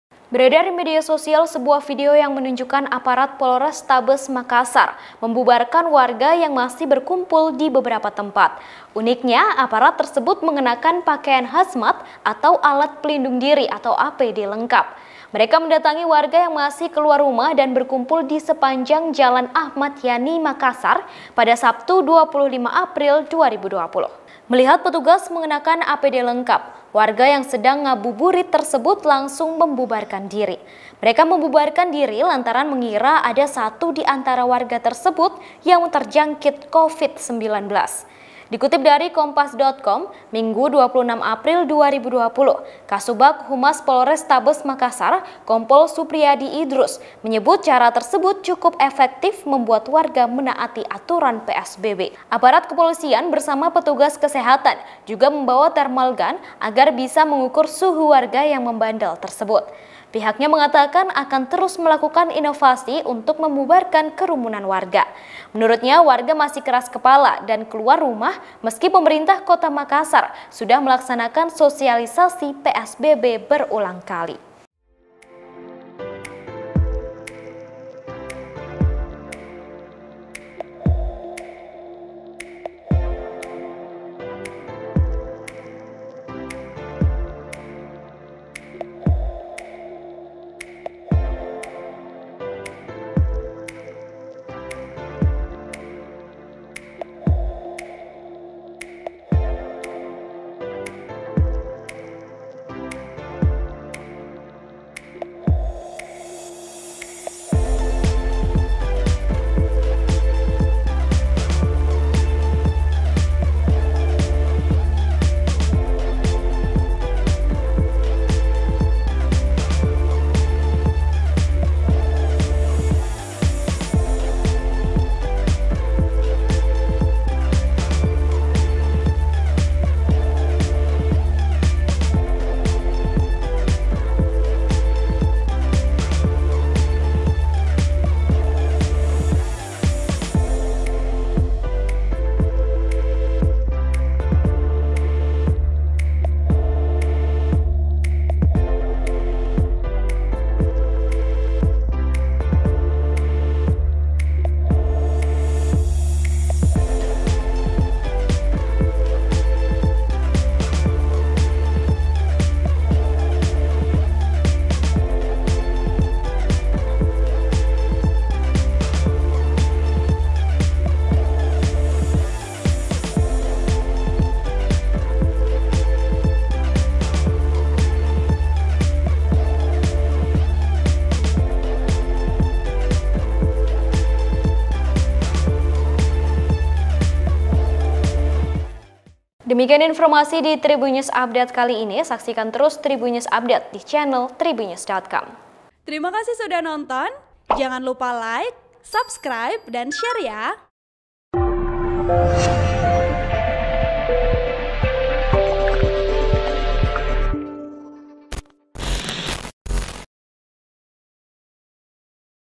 Unik, Polisi di Makassar Bubarkan Kerumunan saat PSBB Pakai Kostum APD Lengkap
Uniknya, aparat tersebut mengenakan pakaian hazmat atau Alat Pelindung Diri (APD) lengkap.

Mereka mendatangi warga yang masih ke luar rumah dan berkumpul di sepanjang jalan Ahmad Yani, Makassar pada Sabtu (25/4/2020).

Melihat petugas mengenakan APD lengkap, warga yang sedang nagbuburit tersebut langsung membubarkan diri.

Mereka membubarkan diri lantaran mengira ada satu di antara warga tersebut yang terjangkit Covid-19.

Dikutip dari Kompas.com, Minggu (26/4/2020), Kasubag Humas Polrestabes Makassar, Kompol Supriady Idrus menyebut cara tersebut cukup efektif membuat warga menaati aturan PSBB.

Aparat kepolisian bersama petugas kesehatan juga membawa thermal gun agar bisa mengukur suhu warga yang membandel tersebut.

"Kalau ada yang membandel langsung kita periksa suhunya karena di dalam patroli itu ada dari petugas kesehatan. Kalau memang ada indikasi Covid maka secepat mungkin langsung kita tindaklanjuti," kata Supriady.

Pihaknya mengatakan, akan terus melakukan inovasi untuk membubarkan kerumunan warga.

Menurutnya warga masih keras kepala dan keluar rumah meski pemerintah Kota Makassar sudah melaksanakan sosialisasi PSBB berulang kali.

(Tribun-Video.com/Kompas.com)

Artikel ini telah tayang di Kompas.com dengan judul "Cara Unik Polisi Bubarkan Kerumunan Saat PSBB di Makassar, Datangi Warga dengan APD Lengkap",
Penulis : Kontributor Makassar, Himawan
Editor : Farid Assifa